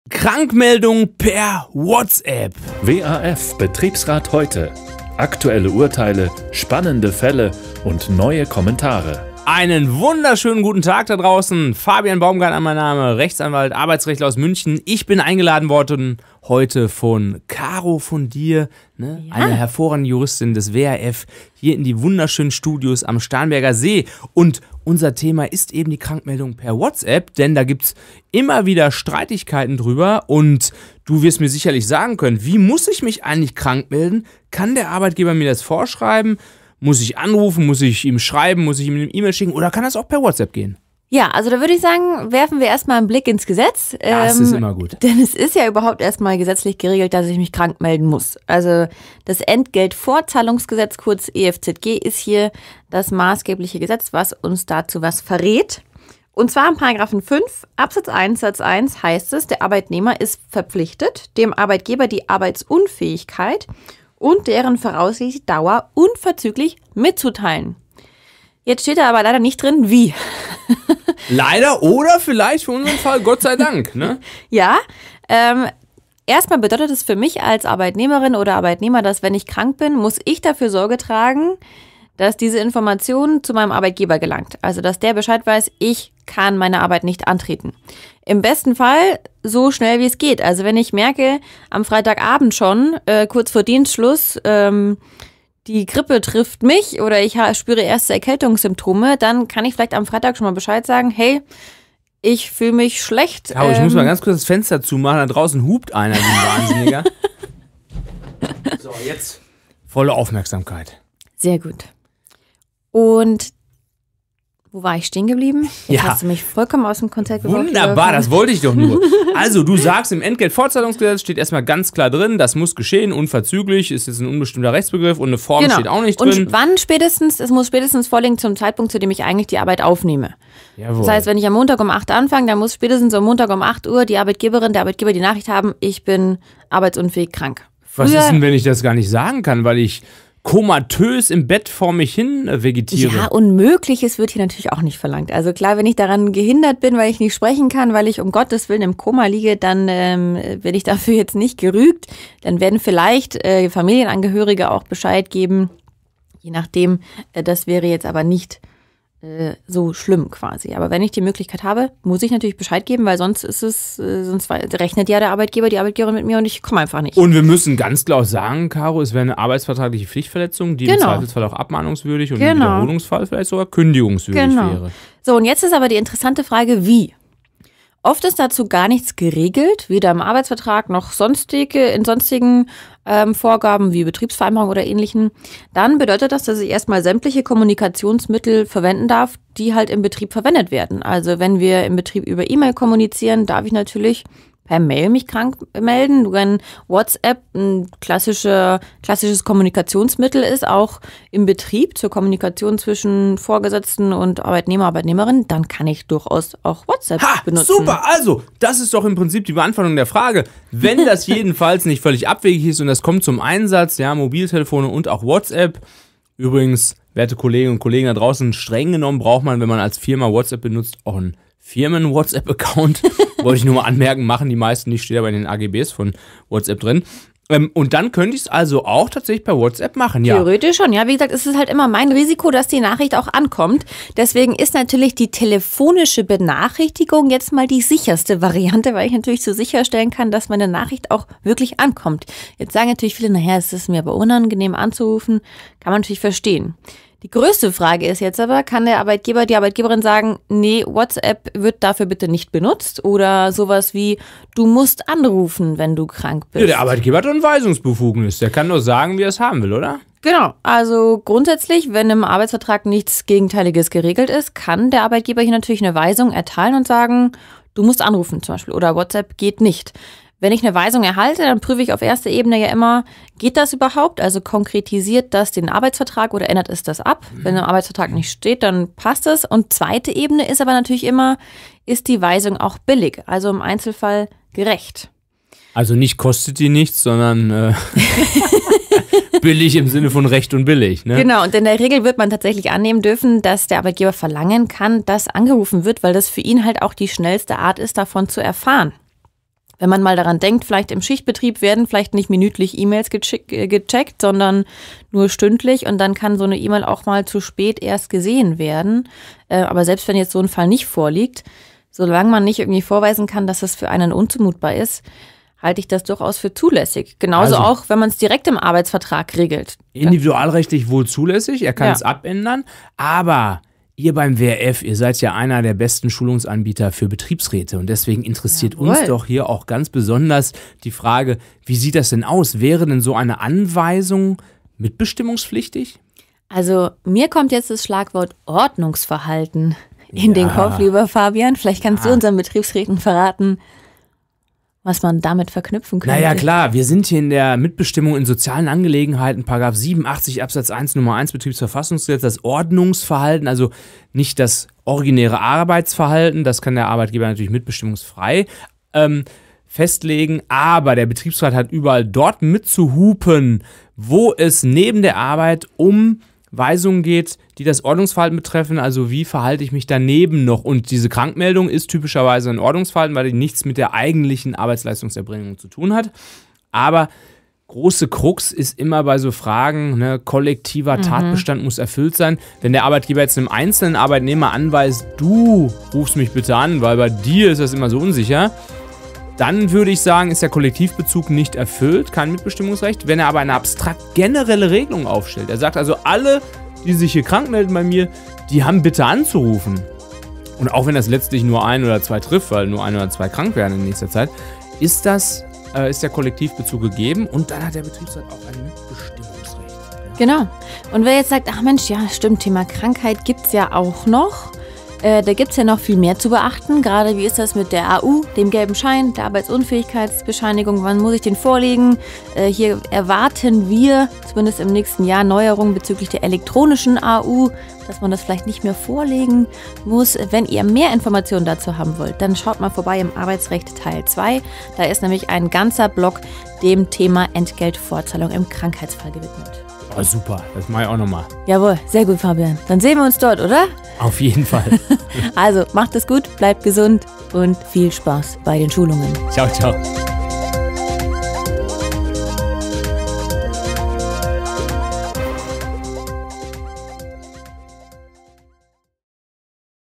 Krankmeldung per WhatsApp?
Krank – und jetzt den Arbeitgeber informieren! Doch wie muss ich mich eigentlich krank melden? Kann der Arbeitgeber mir vorschreiben, dass ich anrufen muss und nicht per WhatsApp schreiben darf? Unsere Rechtsanwälte Carolin Wiesbauer und Fabian Baumgartner klären auf.

Themen in der heutigen Folge:
-Was sagt das Gesetz dazu?
-Kann der AG die Art der Krankschreibung eingrenzen?
-Kann der AG Kommunikationswege verbieten?
-Welche Mitbestimmungsrechte hat der BR?
-Welche Neuerungen gibt es zum Thema Krankheit und Entgeltfortzahlung?

SOCIAL MEDIA

WISSEN & AUSTAUSCH